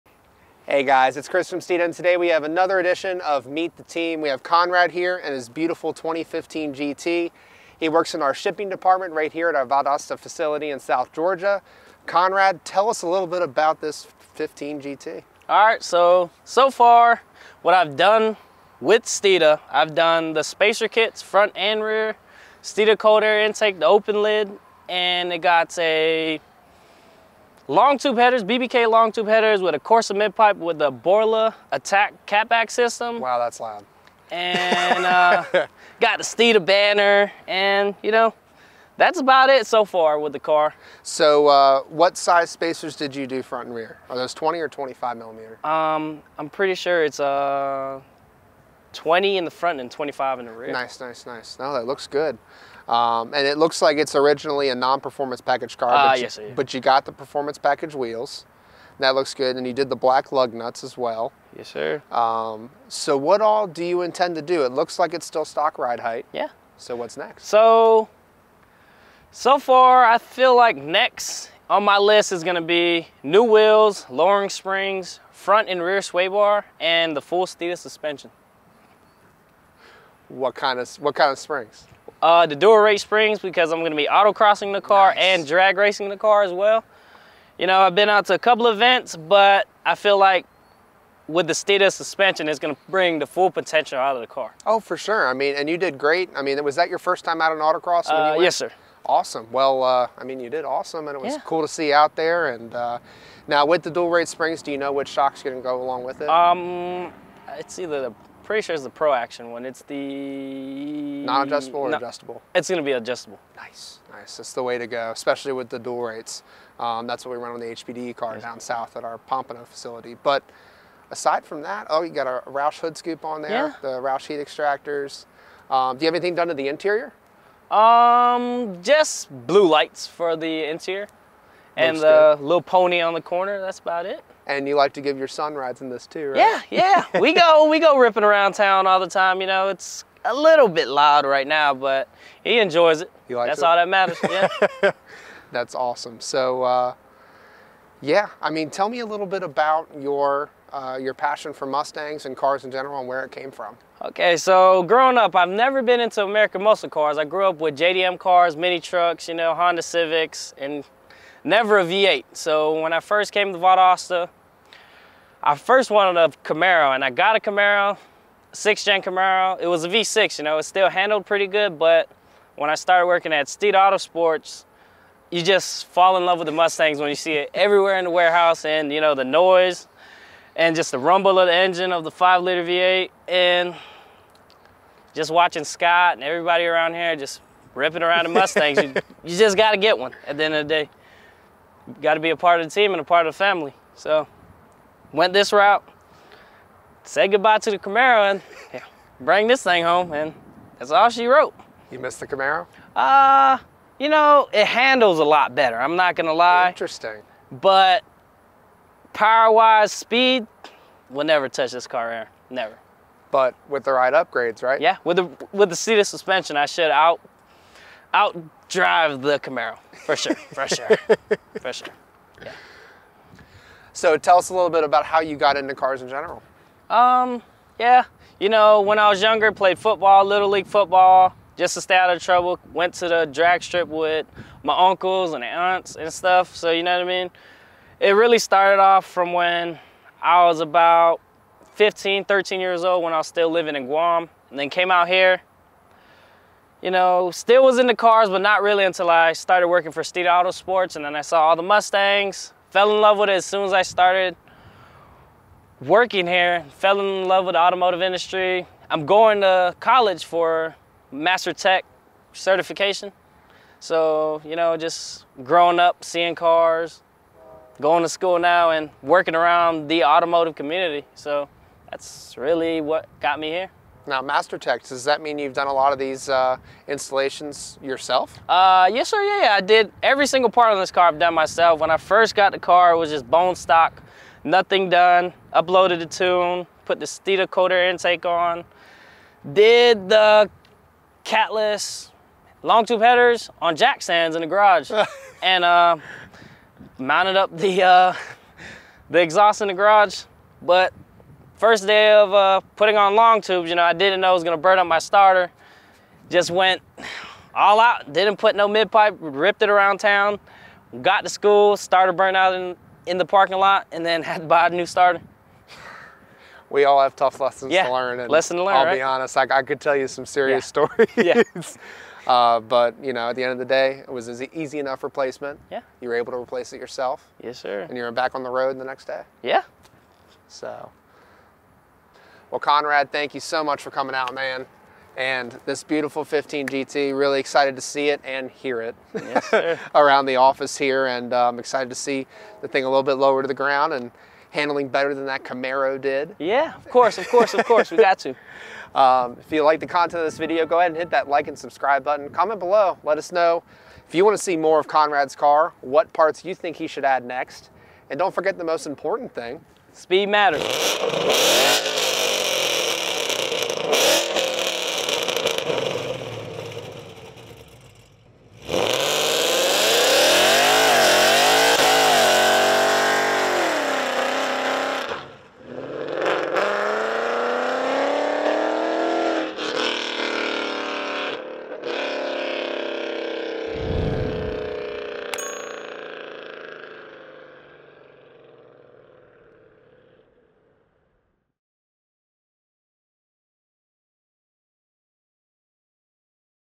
Meet The Team: Conrado Villamanca & His Blue Stallion
You’ve seen the cars, but now it’s time to meet the people who make them possible in our new Meet The Team segment!
Shipping Associate Conrado Villamanca’s first Mustang - a 2015 GT in Deep Impact Blue - is a definite looker, and with some serious volume to it. Coming from JDM to a Camaro and finally, the 2015 GT, Conrado’s car experience covers all of the ground in the industry already.  With big plans for both autocross and drag racing, Conrado’s first muscle car is setting up to perform well above its weight class.